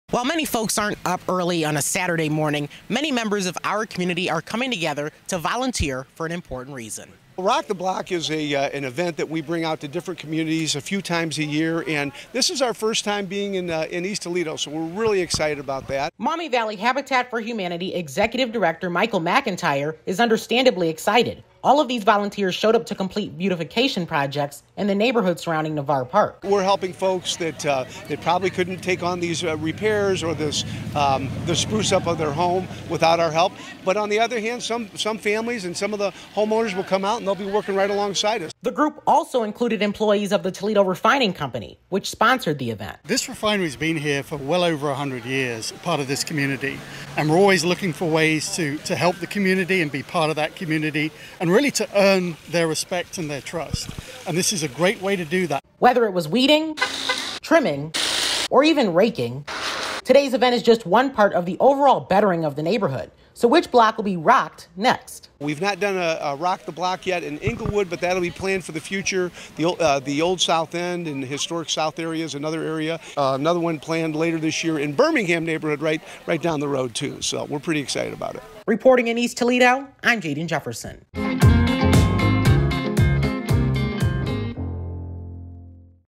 Volunteers 'Rock the Block' in East Toledo, Saturday morning
While many folks aren’t up early on a Saturday morning, many members of our community came together to volunteer for an important reason.